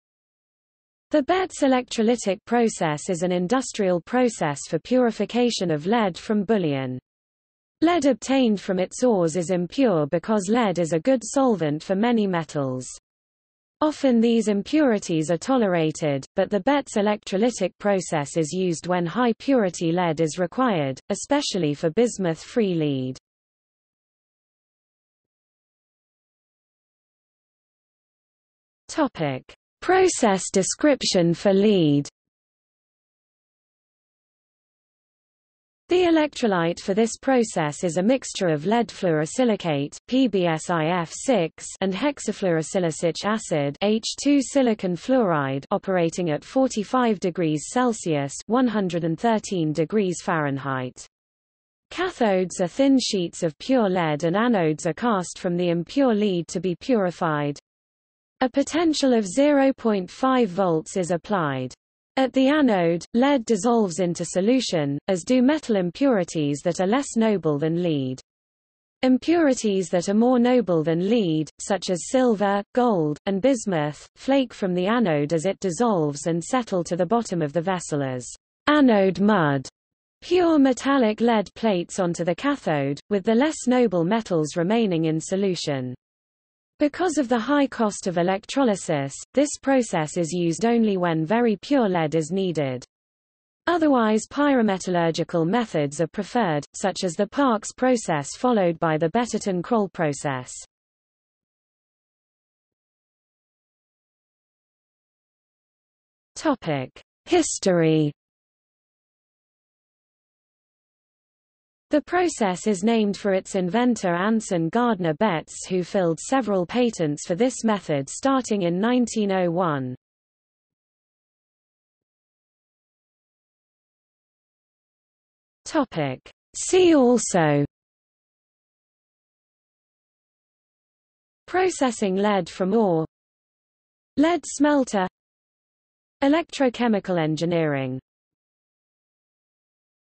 Betts electrolytic process | Wikipedia audio article
00:00:25 1 Process description for lead
00:01:42 2 History
00:02:02 3 See also

- Socrates

SUMMARY
The Betts electrolytic process is an industrial process for purification of lead from bullion.  Lead obtained from its ores is impure because lead is a good solvent for many metals.  Often these impurities are tolerated, but the Betts electrolytic process is used when high purity lead is required, especially for bismuth-free lead.